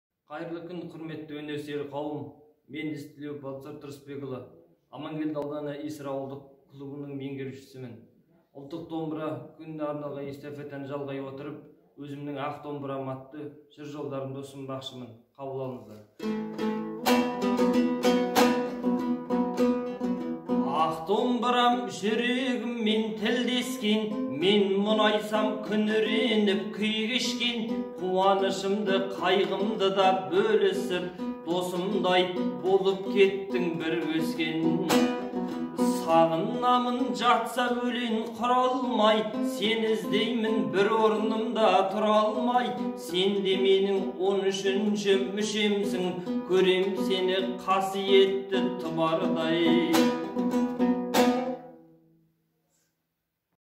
Амангелді ауданының ақыны Балтасар Ізтілеуов
Ұлттық домбыра күні аясында Қостанай облысы ақындарының киелі мұрамыз Домбыраға  арнаған жыр шумақтары